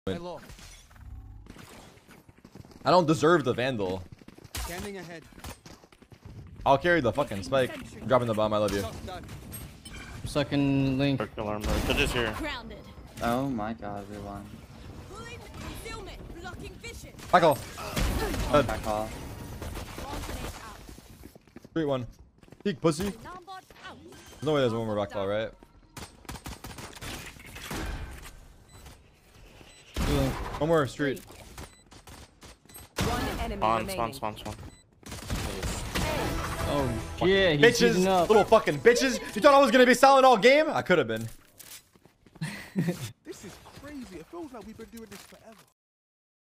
SEN Tarik Shows What Clean RAW AIM Looks Like Ft. Stewie2k
✅ Make Sure To Follow The Creator Of This Clip On The Link Above.

Peace!